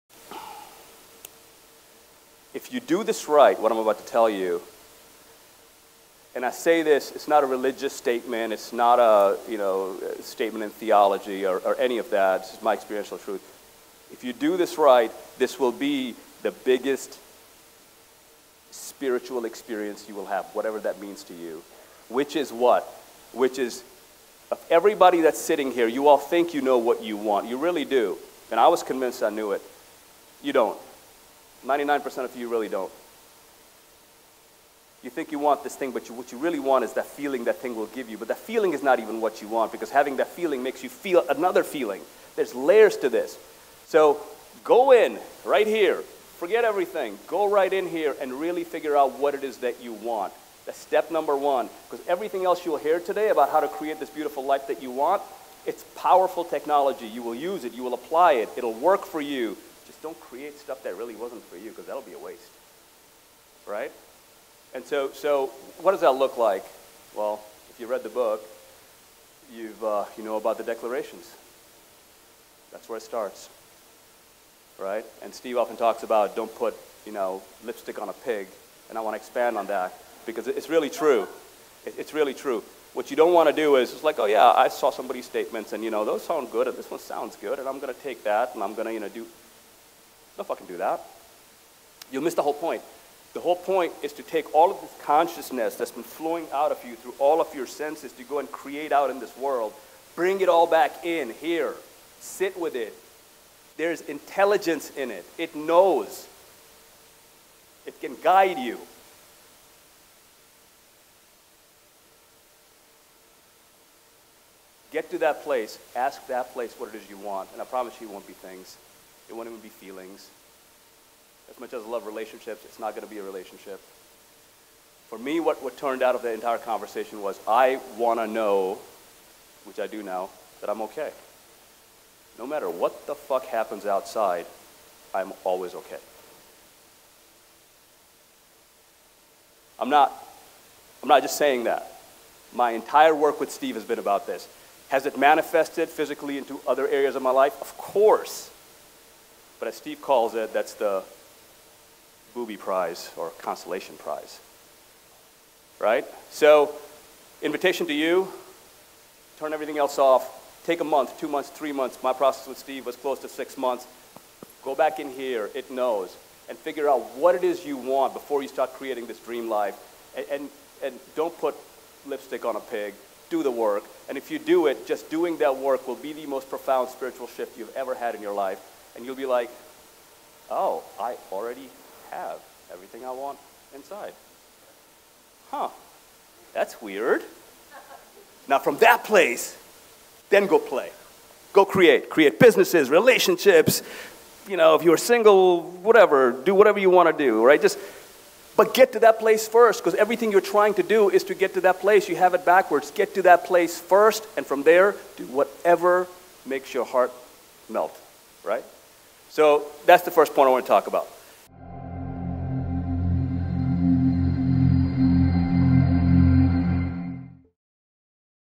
Step #1 To Being The Best You | Karan Limitless Rai
In ￼this video, from The Ultimate Experience London Edition, Karan shares insights of transforming his life that can benefit all of us.